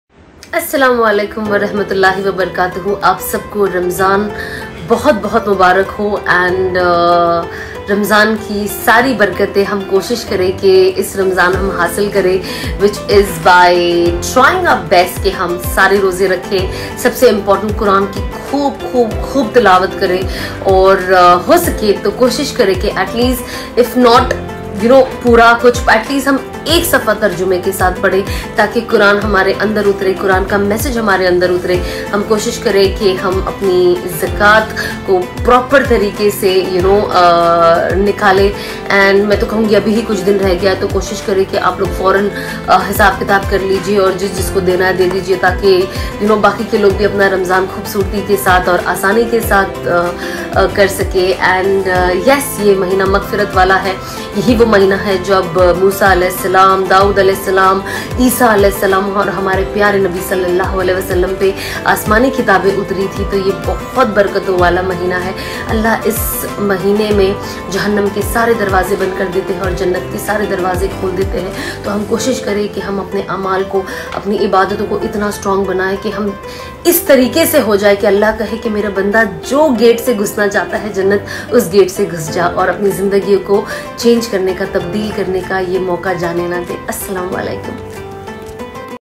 Ramadan Time ♥️sanakhan
Just 3 days n we will all In Sha Allah enter the blessed month of Ramadan 🌙🤲🏻

Let’s make this Ramadan the month of change in our lives and beg Allah for guidance on the straight path.
sana khan ramzan video, sana khan ramzan